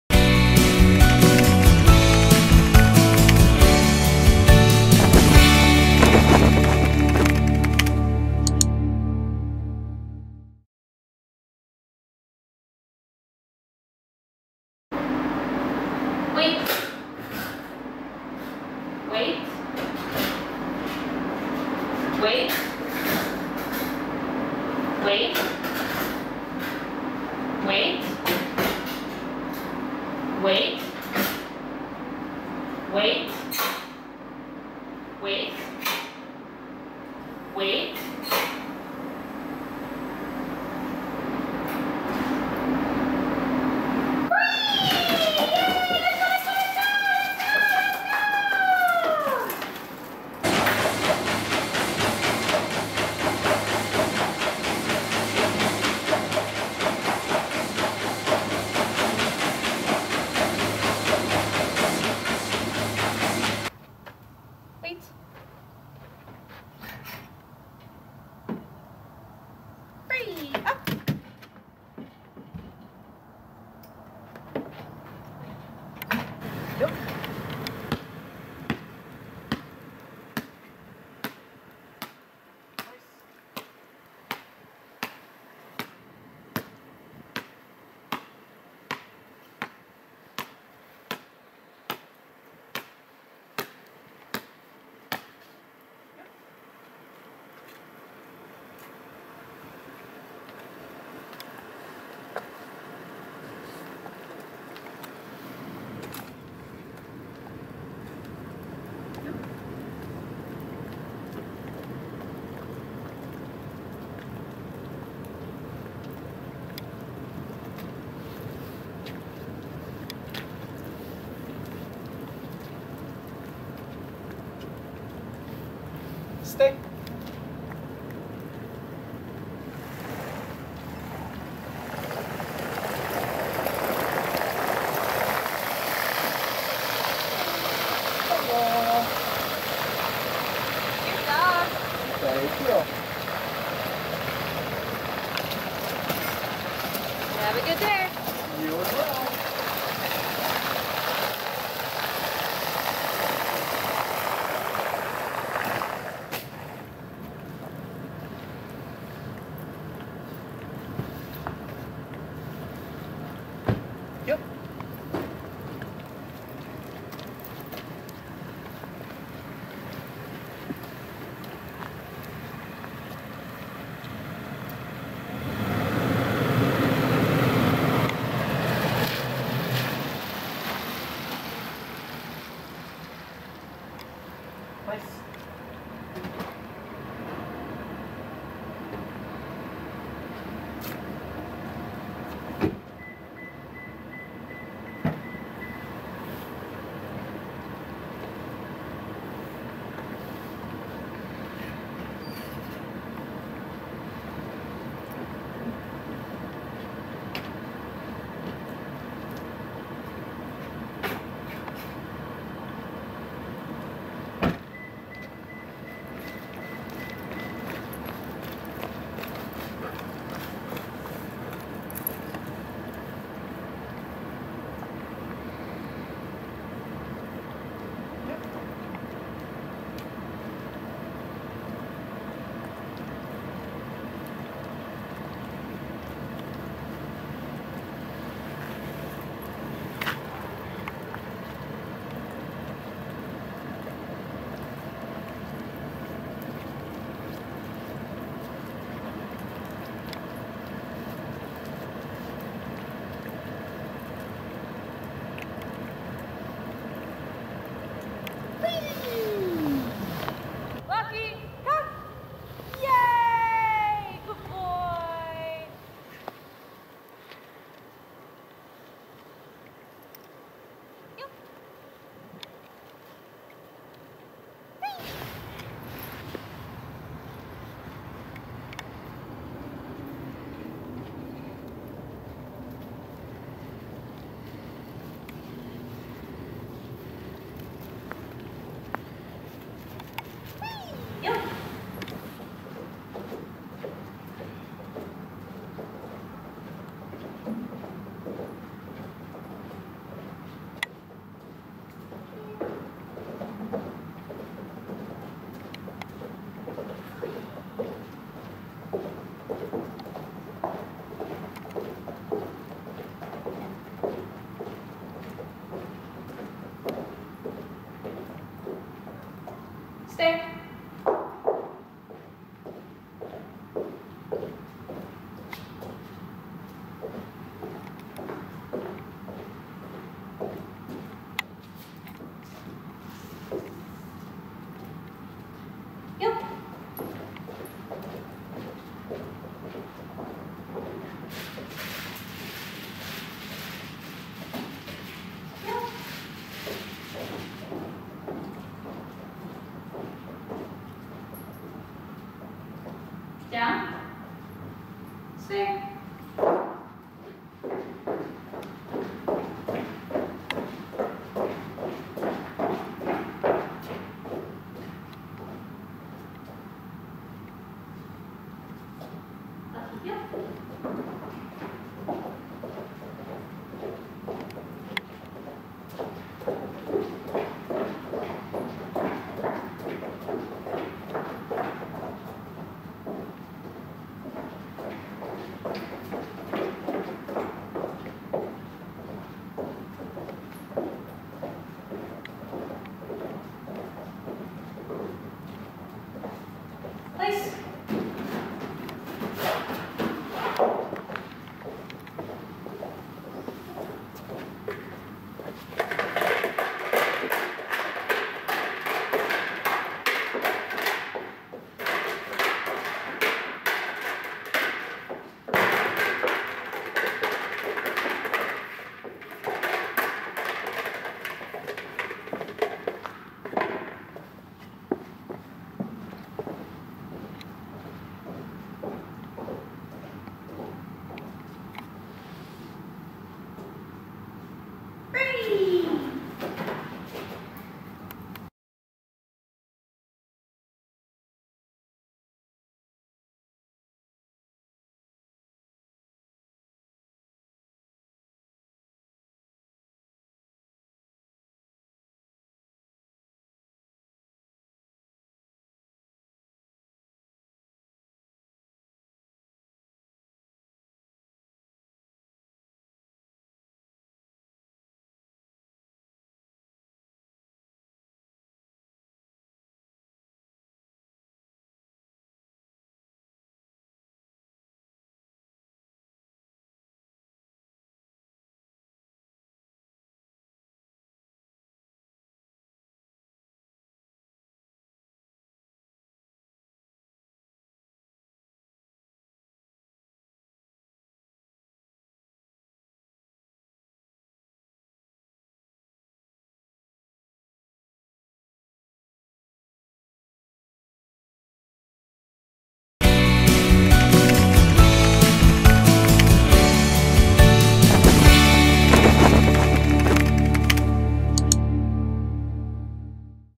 Lucky (Golden Retriever x Great Pyrenees Mix) Boot Camp Dog Training Video Demonstration
Lucky (Golden Retriever x Great Pyrenees Mix) graduated from the Neuman K-9 Academy. During this course dogs learn name-recognition, sit, down, stay, place command, drop-it, walking on-leash without pulling, not jumping on the handler, getting in or out of a vehicle on command, waiting at doors, going into a kennel on command, and socialization with other friendly dogs.